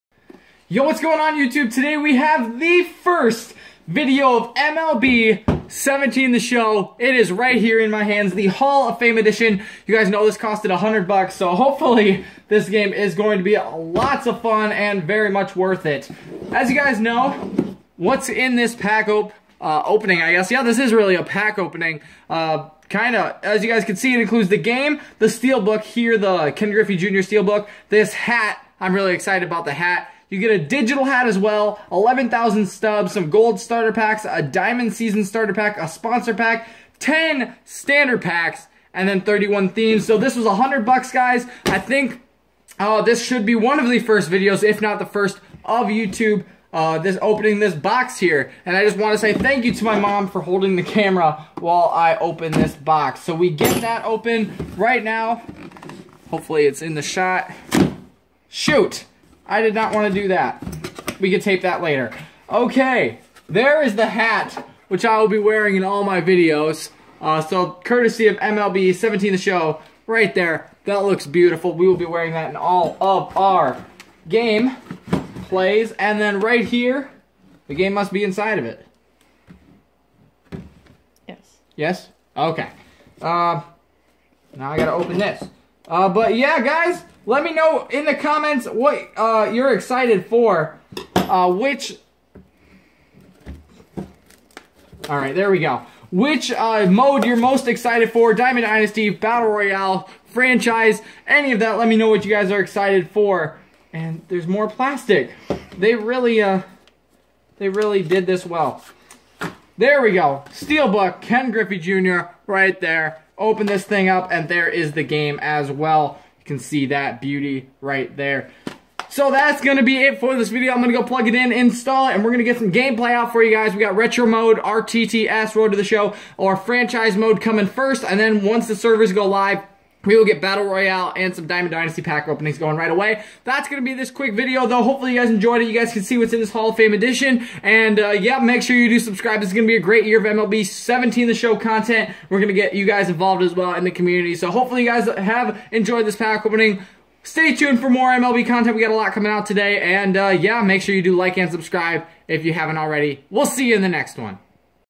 MLB 17 The Show - UNBOXING THE HALL OF FAME EDITION!!!
Welcome to the very first video of MLB 17 The Show on my channel! This is gonna be a great year full of gameplay, commentary, and lots and lots of fun! I can't wait to share this game with you all!!

Thank you to everyone who watches my videos and all my subscribers I can't wait for MLB 17 The Show!

PSN: Clemhawks

-Clemhawks